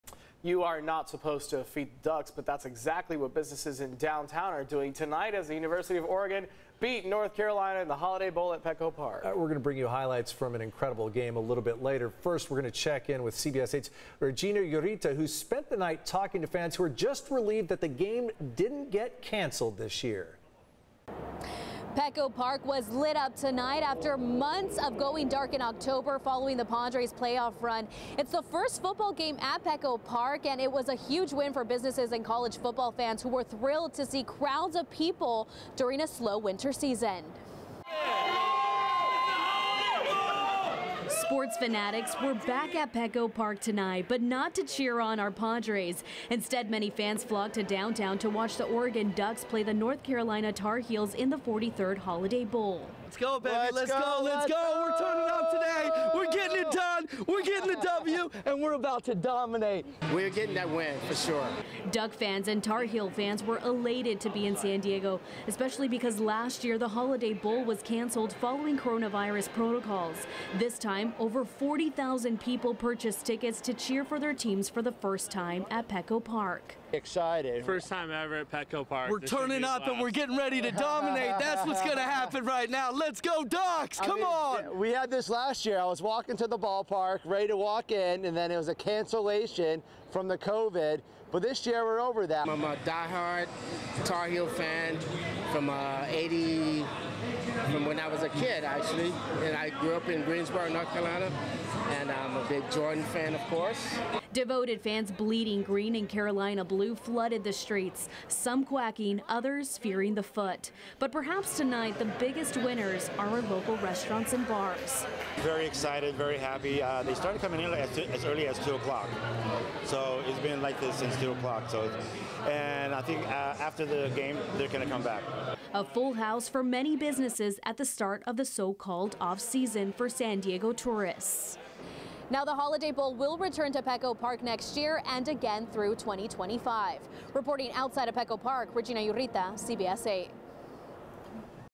Football makes Petco Park debut with Holiday Bowl game
The game brought San Diego football fans as well as fans of the teams from Oregon and North Carolina to downtown San Diego.